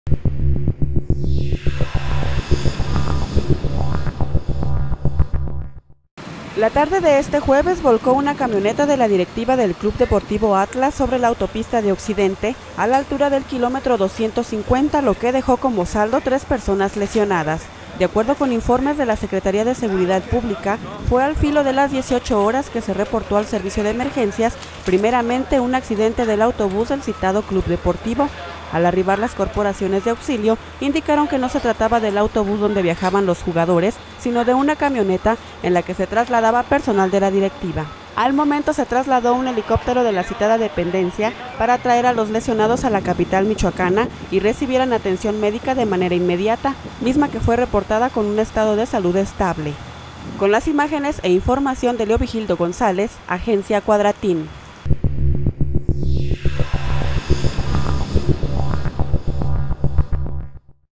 Volcadura deja heridos a integrantes del Atlas
MORELIA, Mich. 15 de agosto de 2013.- Tres lesionados fue el saldo de una volcadura de una camioneta del club deportivo Atlas registrada la tarde de este jueves sobre la Autopista de Occidente a la altura del kilometro 250, entre los que se encuentran el secretario Técnico del equipo. Los hechos se registraron al filo de las 18:00 horas de este jueves, cuando personal de Protección Civil (PC) del Estado fueron alertados de que en citado lugar se había registrado un aparatoso accidente automovilístico donde estaba involucrado un vehículo del citado equipo de fútbol soccer profesional. Los paramédicos al arribar al lugar confirmaron los hechos por lo que pidieron el apoyo de una aeronave de la Secretaría de Seguridad Pública (SSP)  para trasladar a los lesionados a la capital michoacana con la finalidad de que recibieran atención médica.